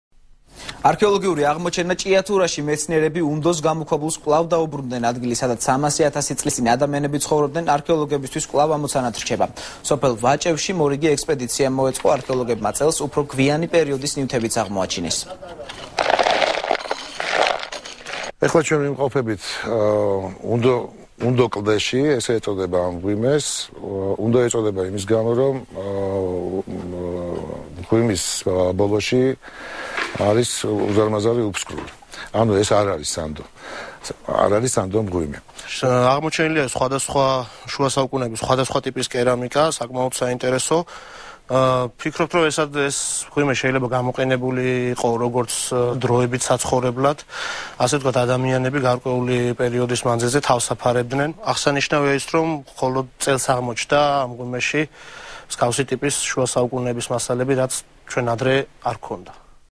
ახალი 12 | არქეოლოგიური აღმოჩენა | 15.08.12
არქეოლოგიური აღმოჩენა ჭიათურაში. მეცნიერები უნდოს გამოქვაბულს კვლავ დაუბრუნდნენ. ადგილი, სადაც სამასი ათასი წლის წინ ადამიანები ცხოვრობდნენ, არქეოლოგებისთვის კვლავ ამოცანად რჩება. სოფელ ვაჭევში მორიგი ექსპედიცია მოეწყო. არქეოლოგებმა წელს, უფრო გვიანი პერიოდის ნივთებიც აღმოაჩინეს.